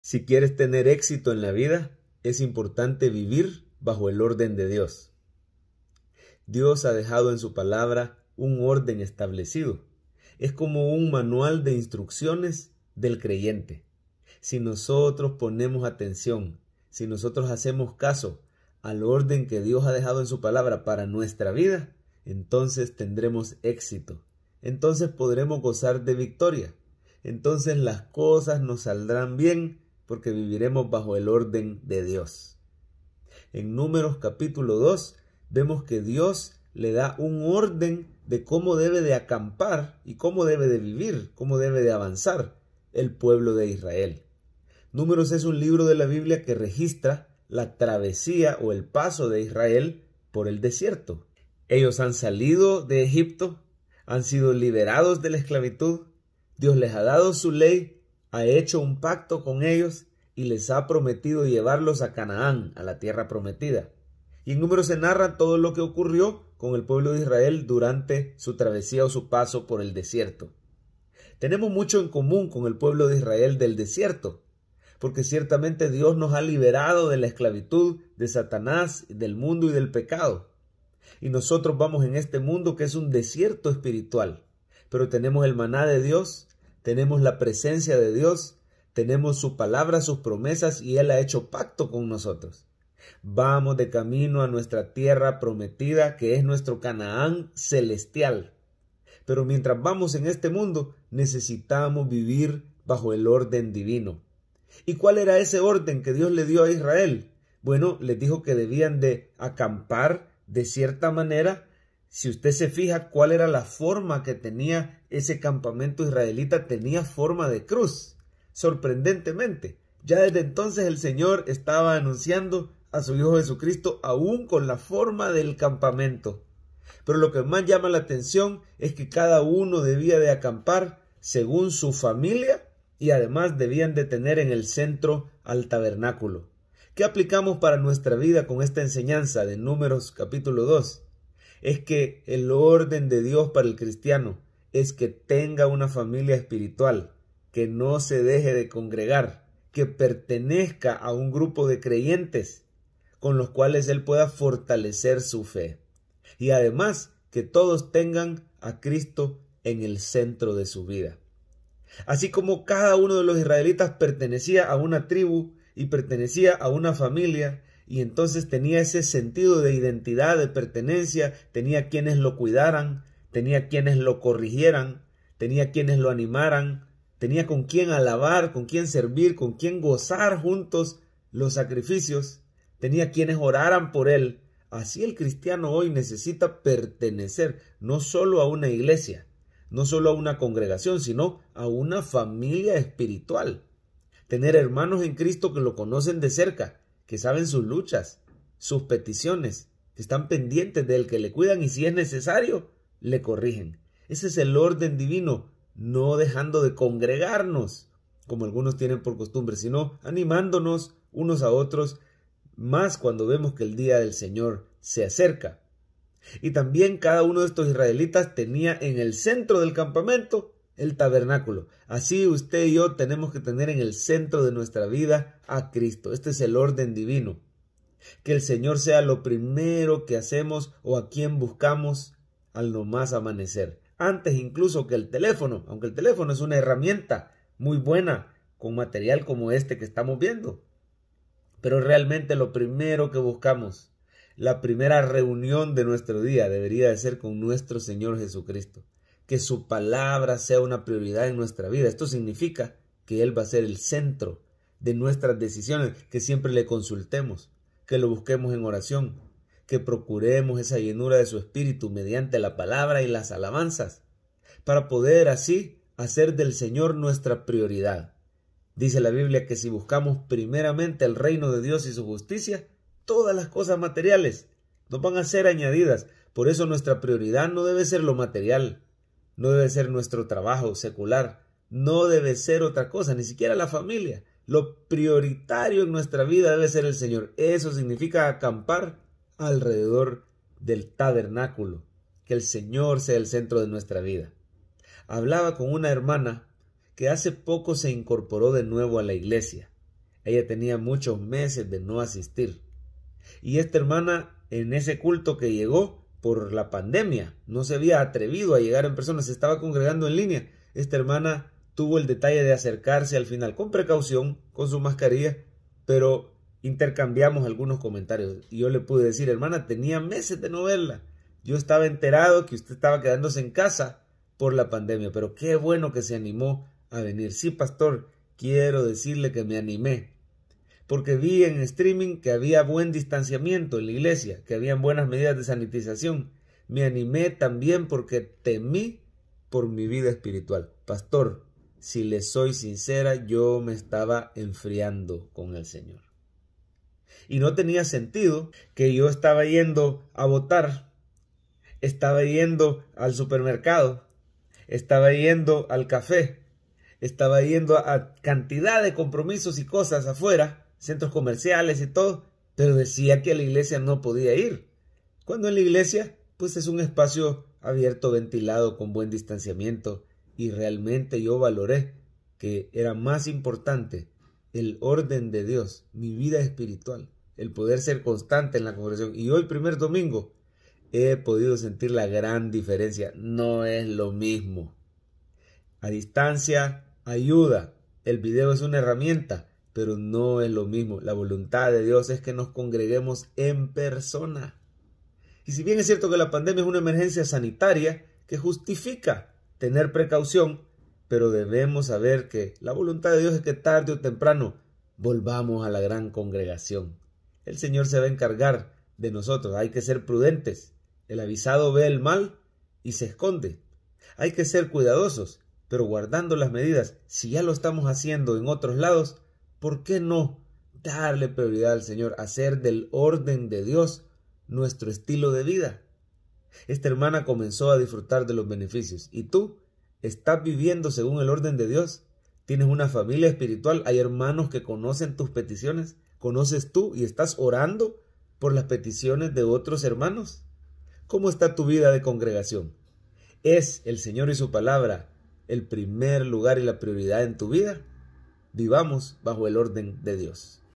El Orden de Dios para tu vida | Números 2
En nuestro paso por esta vida nos irá mejor si atendemos el orden establecido por Dios.

¿Quieres caminar en bendición? Conozcamos el Orden de Dios. Comparte SabiduríaParaLaVida.